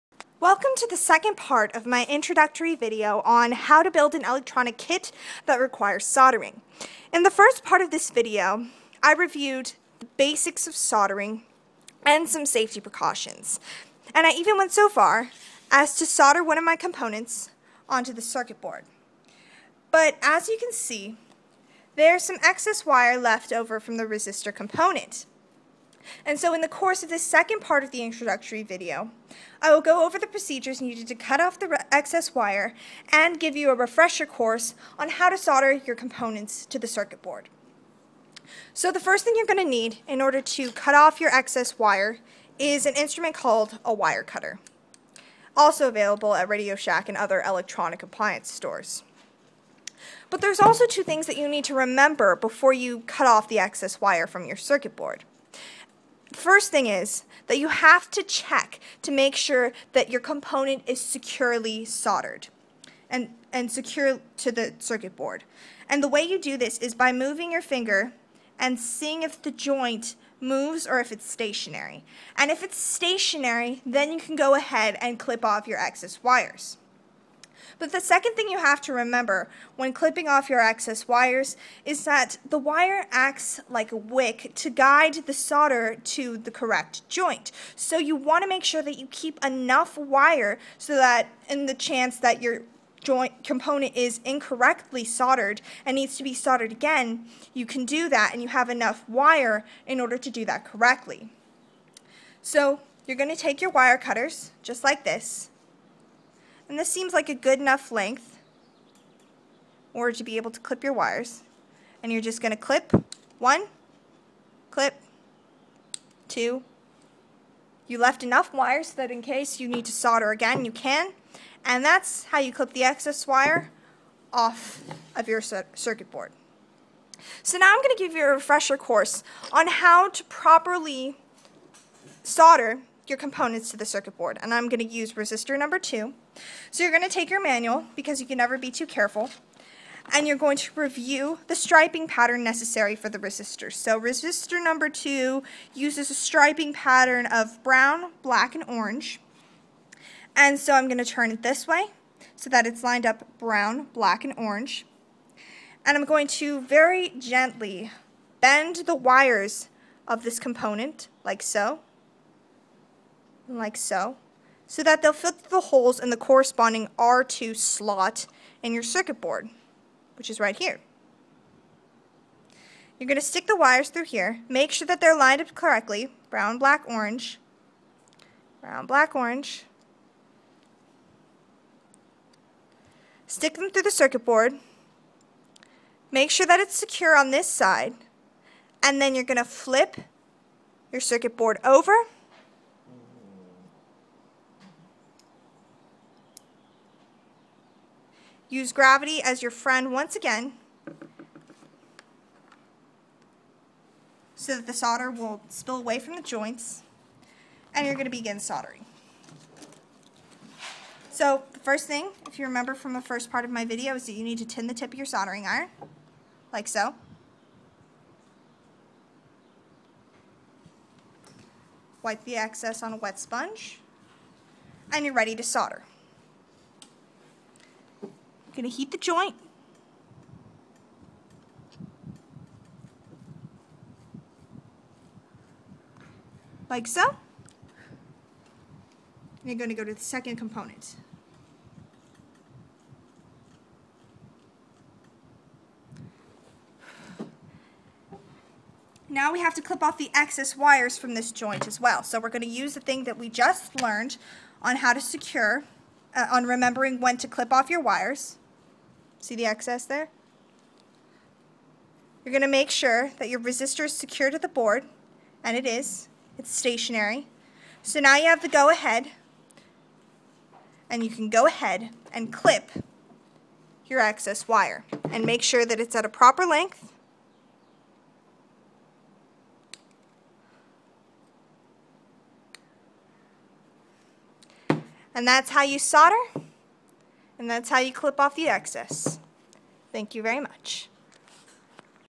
Velleman Led Light Organ MK 186 Soldering Instructions for
Basic soldering instructions for novices regarding, what is soldering,how to solder and safety precautions